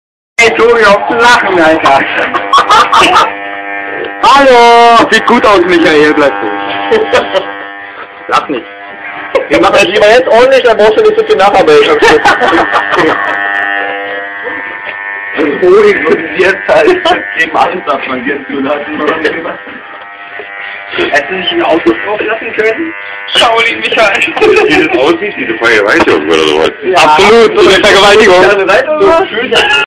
Micha von PLACENTA wird beschnitten 1
Micha von PLACENTA kriegt nen Haarschnitt bei Sven's Geburtstagsparty